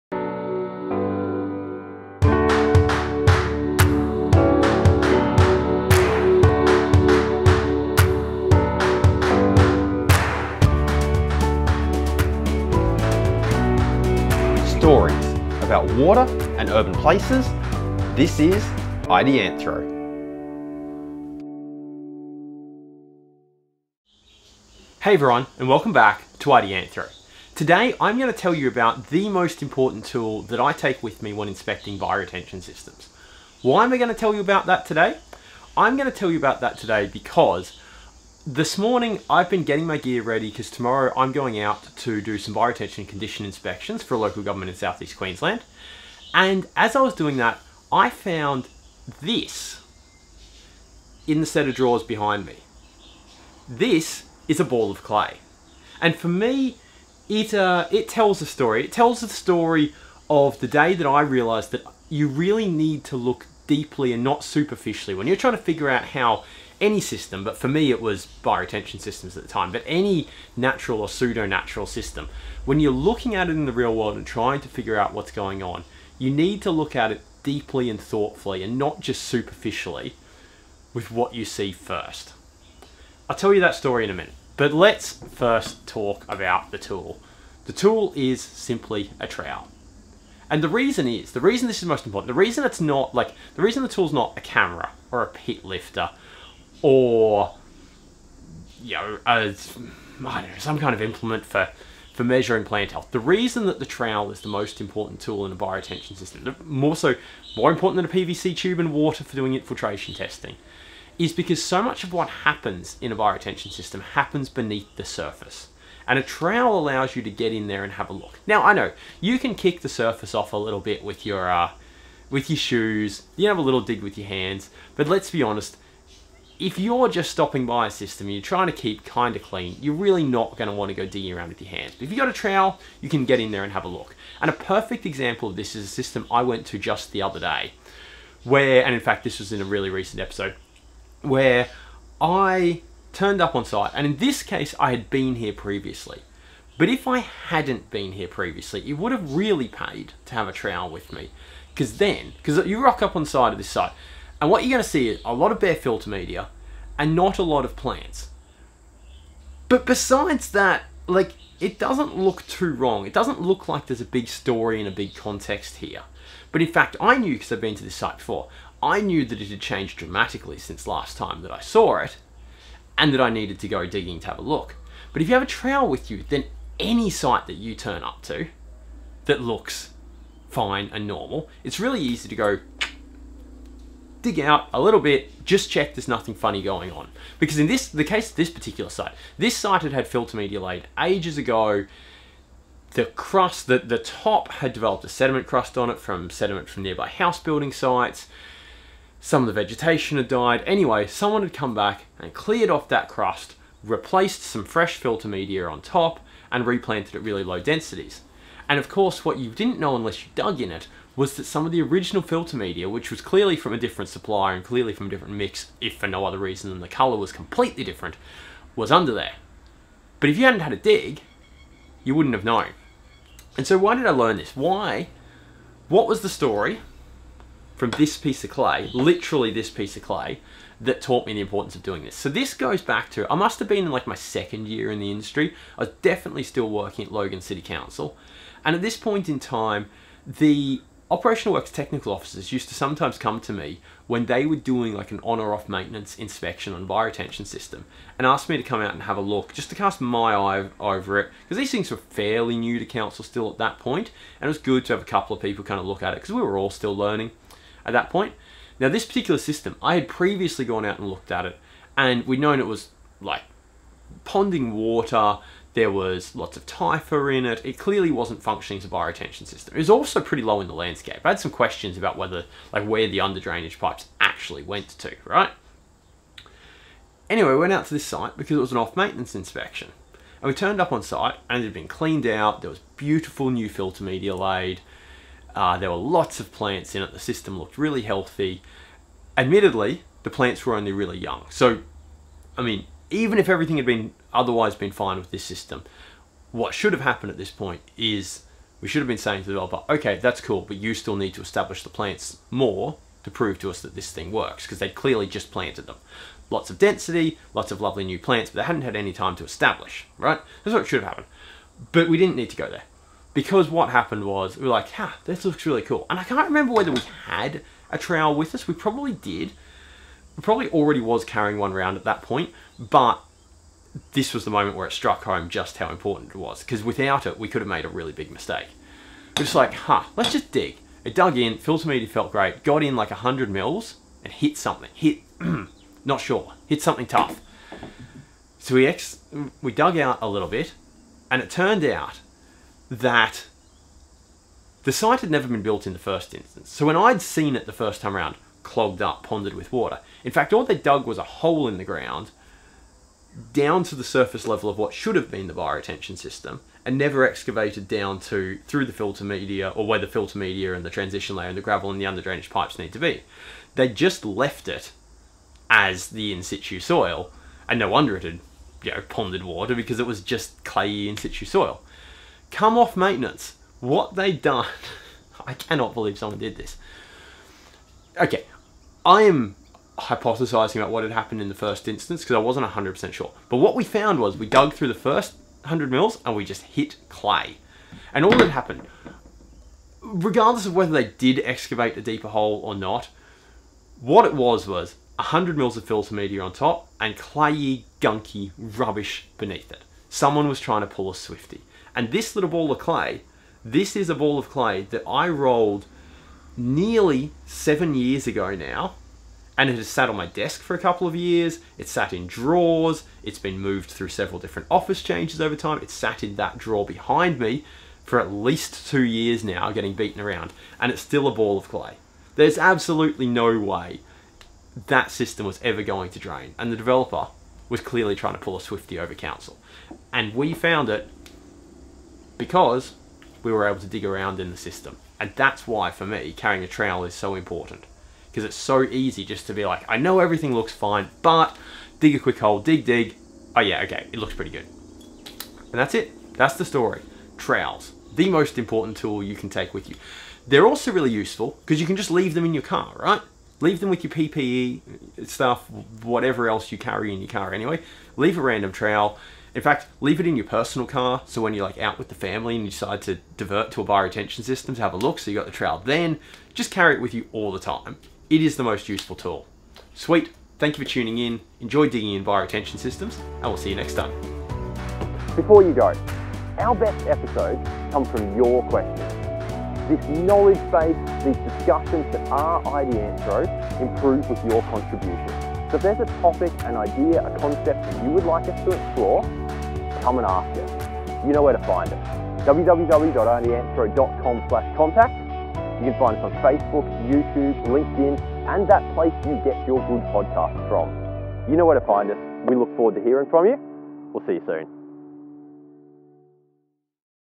Ideanthro Ep 226 - The most important tool for inspecting bioretention systems
Want it in audio?